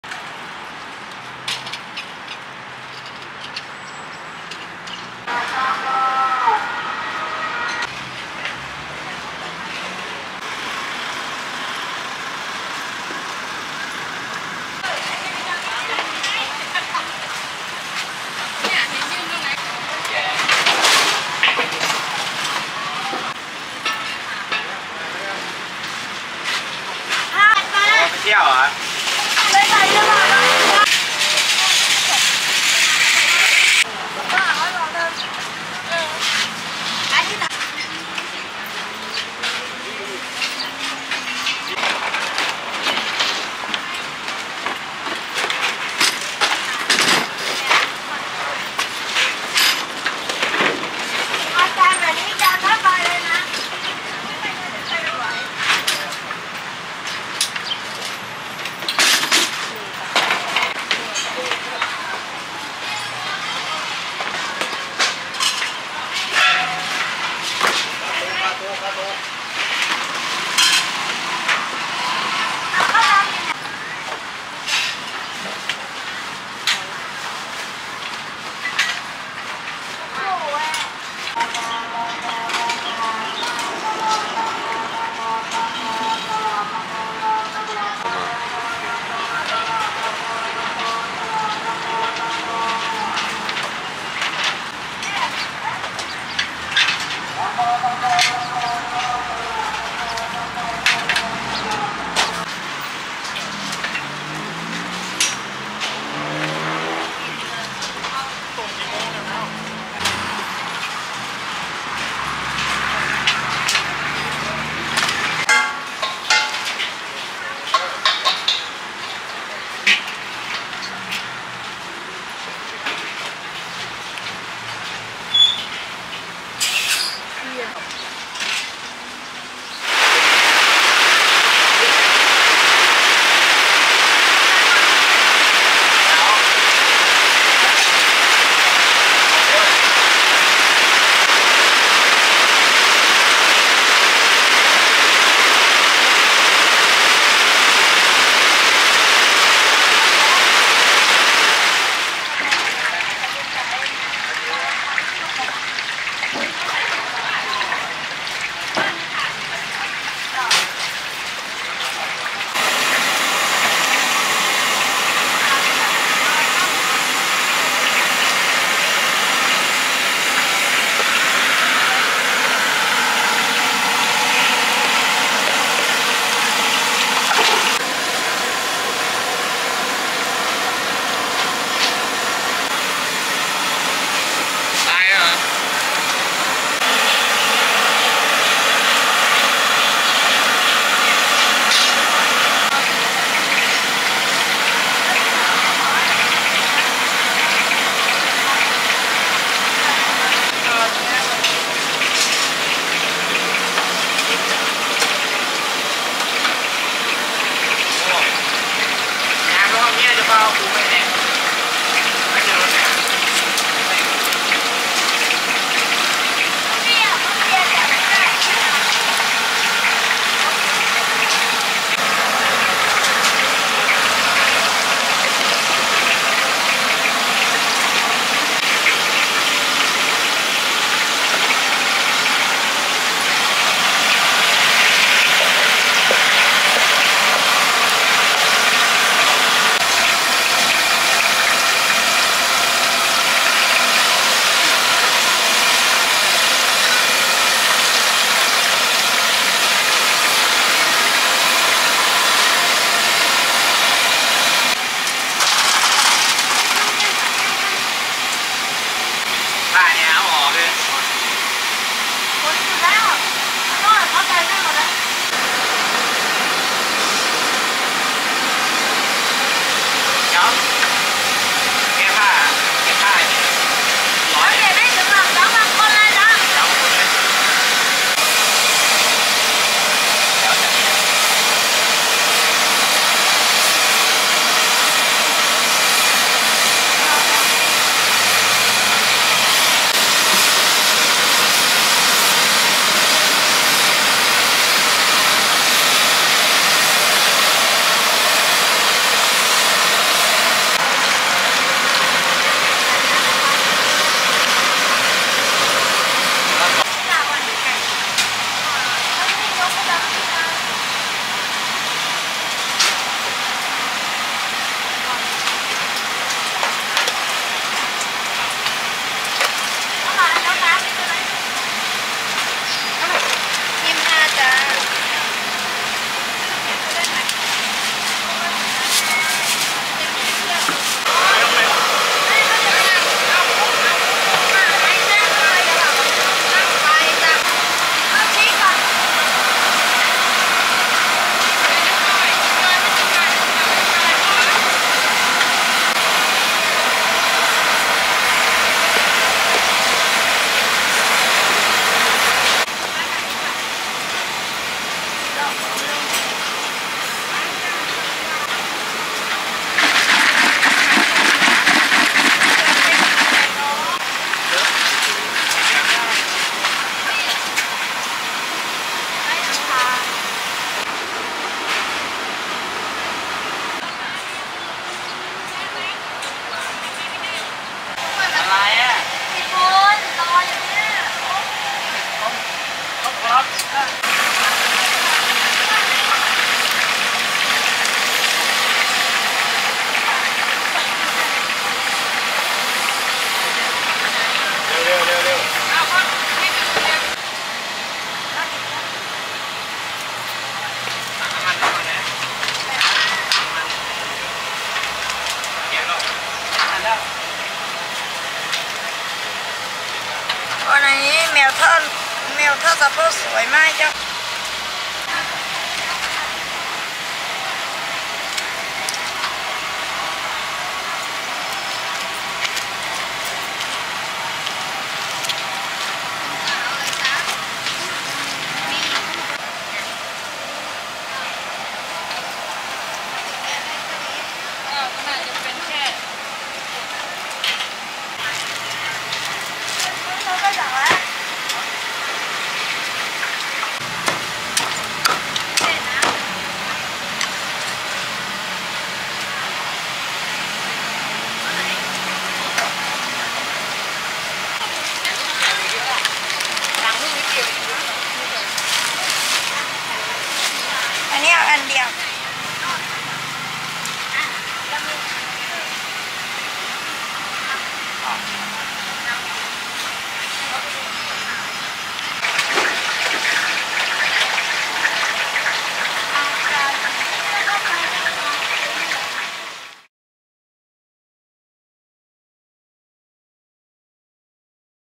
Popular Fried Chicken and Rice! $0.75 Cheap and Delicious | Thai Street Food
Thank you so much for taking the time to watch my video. Your support means the world to me! Thanks again for your support 🙏🏻❤️